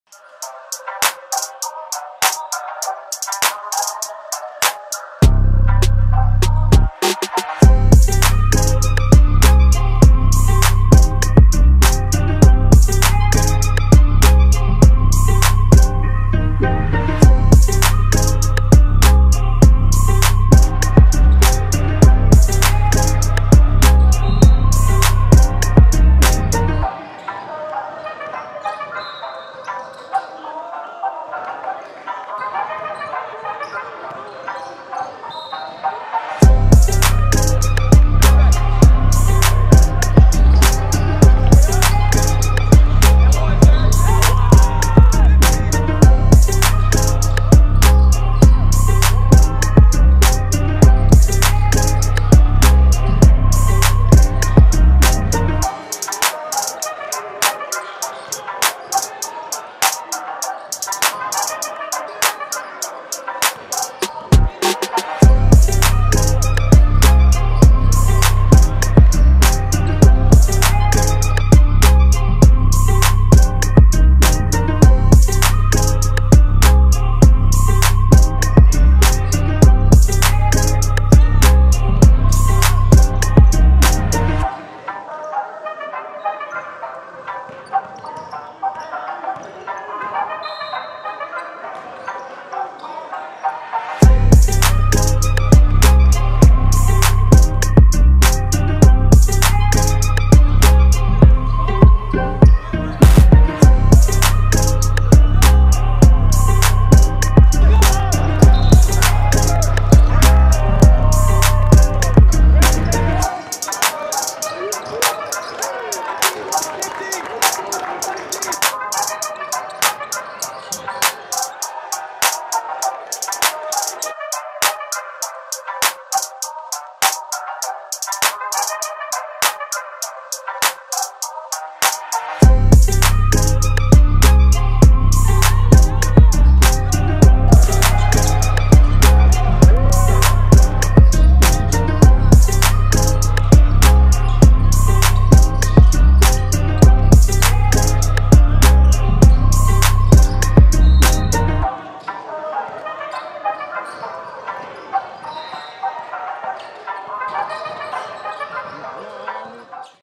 15岁郇斯楠AAU第一阶段个人高光！Alabama offer！| Sinan Huan (2007-214cm-PF/C) AAU 3SSB Highlights Pt.1
Sinan Huan - Class of 2026 - 7 footer!
Joe Tsai Basketball Scholarship
来自中国的蔡崇信篮球奖学金